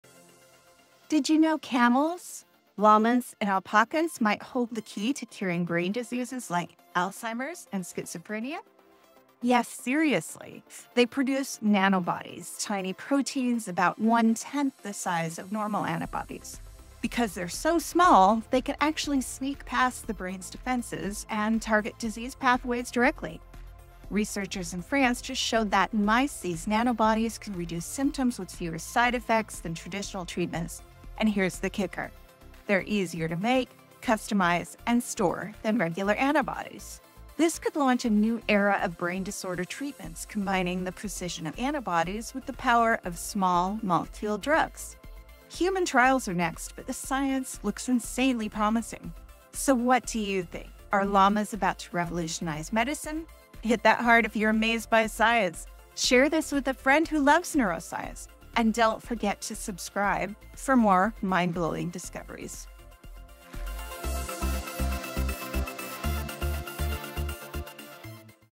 Alpacas to the Rescue: The Future of Brain Health
How Camels, Llamas, and Alpacas Could Cure Brain Diseases!
Discover how camels, llamas, and alpacas are producing tiny proteins called nano bodies that could revolutionize the treatment of brain diseases like Alzheimer's and schizophrenia. Researchers in France have shown promising results in mice, and human trials are next. Learn about the potential of these nano bodies to provide more precise treatments with fewer side effects and easier production and storage compared to traditional antibodies.
00:00 Unlocking the Secrets of Brain Diseases with Camels, Llamas, and Alpacas
00:10 The Power of Nano Bodies
00:24 Promising Research and Future Prospects
00:49 Join the Conversation
00:53 Stay Tuned for More Discoveries